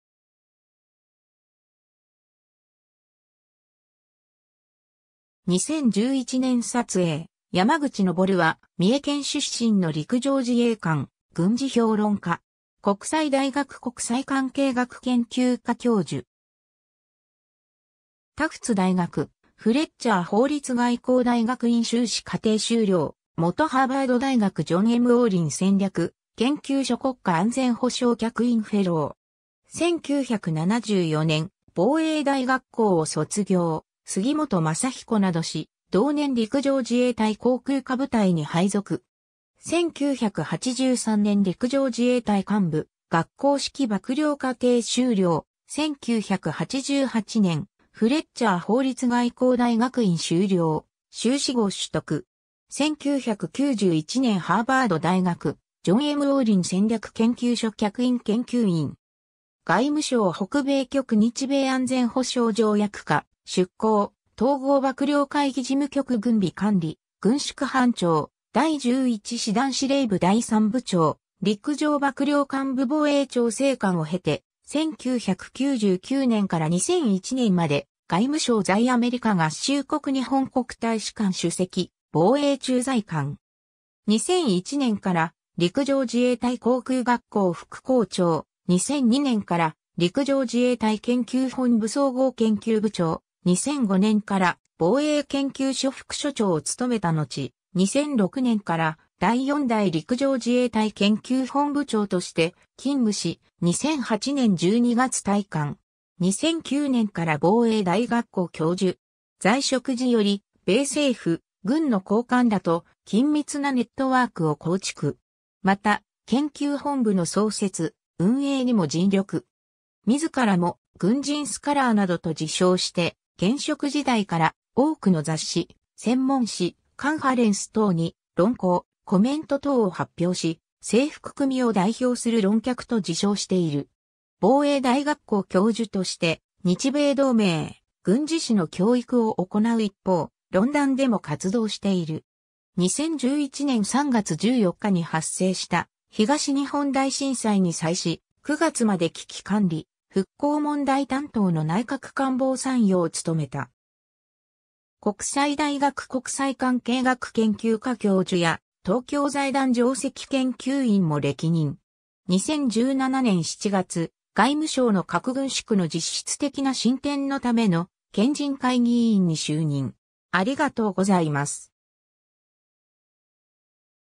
山口昇 (軍事評論家)
2011年撮影 山口 昇（やまぐち のぼる 1951年（昭和26年） - ）は、三重県出身の陸上自衛官・軍事評論家（最終階級は陸将）。
国際大学国際関係学研究科教授。
タフツ大学フレッチャー法律外交大学院修士課程修了、元ハーバード大学ジョン・M・オーリン戦略研究所国家安全保障客員フェロー。
 1974年、防衛大学校を卒業（第18期・同期に火箱芳文（第31代陸上幕僚長）、杉本正彦（第30代海上幕僚長）など）し、同年陸上自衛隊航空科部隊に配属。
1983年陸上自衛隊幹部学校指揮幕僚課程修了、1988年フレッチャー法律外交大学院修了、修士号取得。
1991年ハーヴァード大学ジョン・M・オーリン戦略研究所客員研究員。
 外務省北米局日米安全保障条約課出向、統合幕僚会議事務局軍備管理・軍縮班長、第11師団司令部第3部長(作戦担当)、陸上幕僚監部防衛調整官を経て、1999年から2001年まで外務省在アメリカ合衆国日本国大使館首席防衛駐在官（外務事務官・在米国日本大使館付参事官（国防・陸軍担当））。
2001年から陸上自衛隊航空学校副校長、2002年から陸上自衛隊研究本部総合研究部長、2005年から防衛研究所副所長を務めたのち、2006年から第4代陸上自衛隊研究本部長として勤務し、2008年12月退官。
2009年から防衛大学校教授。
 在職時より、米政府・軍の高官らと緊密なネットワークを構築。
また、研究本部の創設・運営にも尽力。
自らも「軍人スカラー(warrior scholar)」などと自称して、現職時代から多くの雑誌・専門誌・カンファレンス等に論稿・コメント等を発表し、制服組を代表する論客と自称している。
防衛大学校教授（防衛学教育学群安全保障・危機管理教育センター長）として日米同盟、軍事史の教育を行う一方、論壇でも活動している。
 2011年3月14日に発生した東日本大震災に際し、9月まで危機管理・復興問題担当の内閣官房参与を務めた。
国際大学国際関係学研究科教授や、東京財団上席研究員も歴任。
 2017年7月、外務省の核軍縮の実質的な進展のための賢人会議委員に就任。